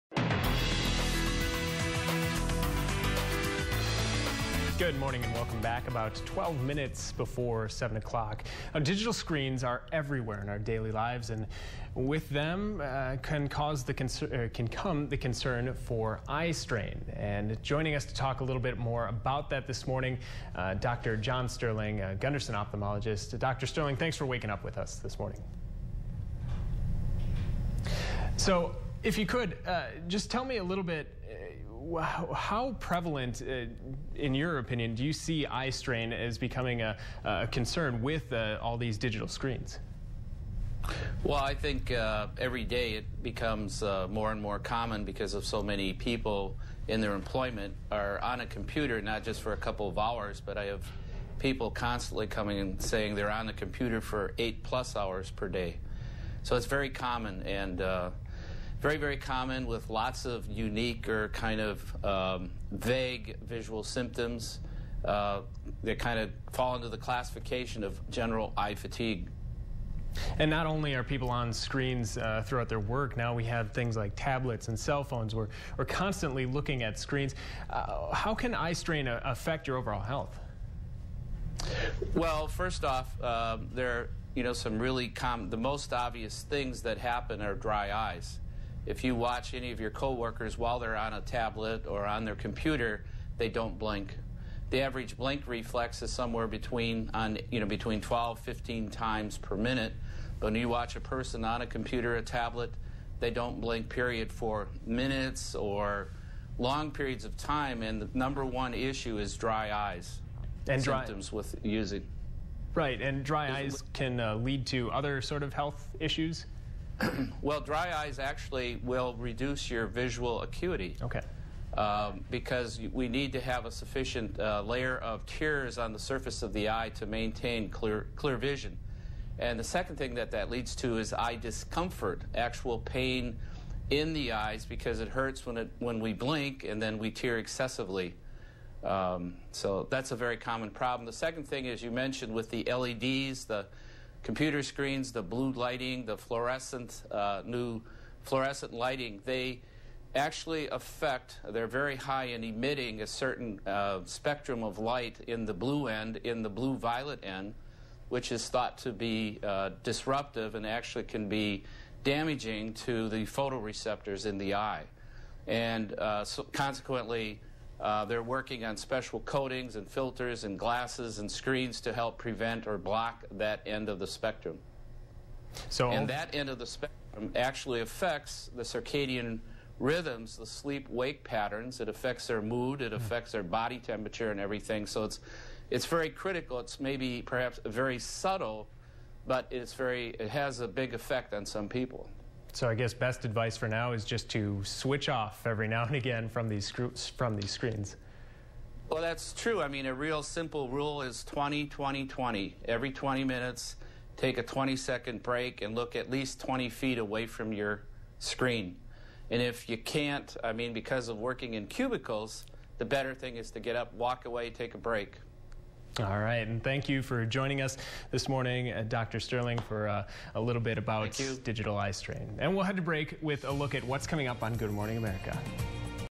John Sterling, OD, Ophthalmology, discusses eye strain from screens
3/24

Learn more about John Sterling, OD, Ophthalmology, on his provider profile